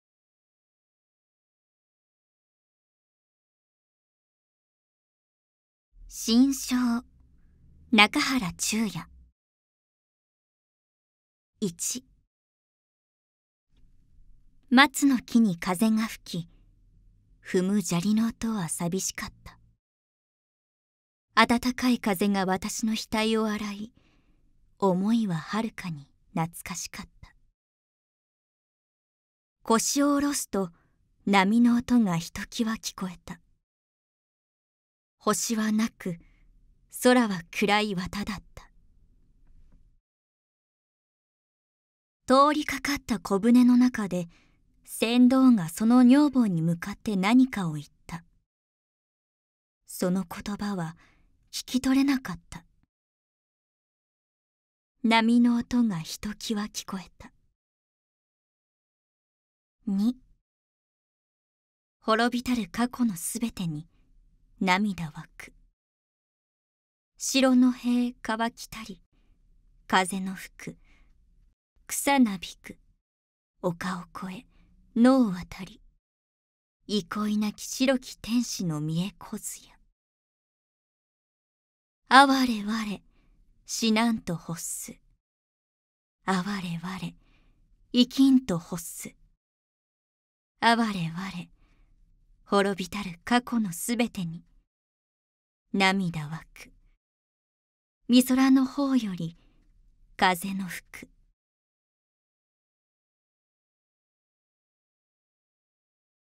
【和の音】心象 中原中也 朗読：鷲見友美ジェナ【朗読】
心象
中原中也
朗読：鷲見友美ジェナ

※和の音とは
日本語の良さ、日本文学の良さを届けるためにLeidenschaftプロデュースが企画した作品群。
声優による日本文学の朗読に日本の風景を撮影した写真を合わせたもの。